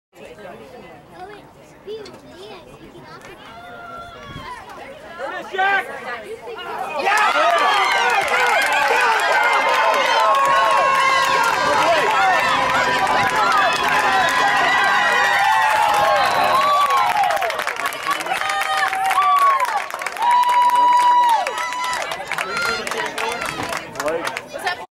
Tipped Catch like Odell Beckham Jr., Dez Bryant, Corey Coleman
Hi, Im Tricia Grace. This is a football video of my son Blake catching a tipped pass - deflected pass like Odell Beckham Jr., Dez Bryant, and his mentor Corey Coleman who plays for Baylor University, and went to JJ Pearce  High School.